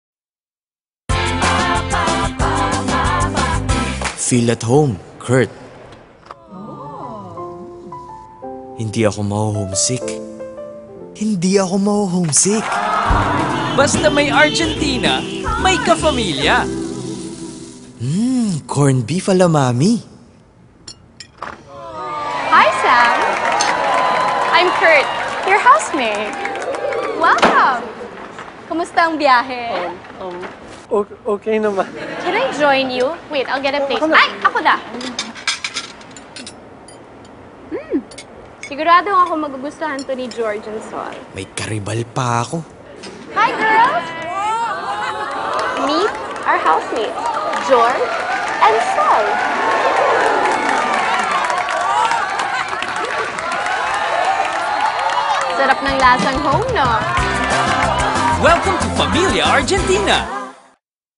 Familia Argentina (starring: Anne Curtis,Solenn & Georgina Wilson)
Argentina Corned Beef Newest Commercial
Starring: Anne Curtis-Smith( @annecurtissmith) Georgina Wilson (@iloveGEORGINA) & Solenn Heussaff (@solennheussaff)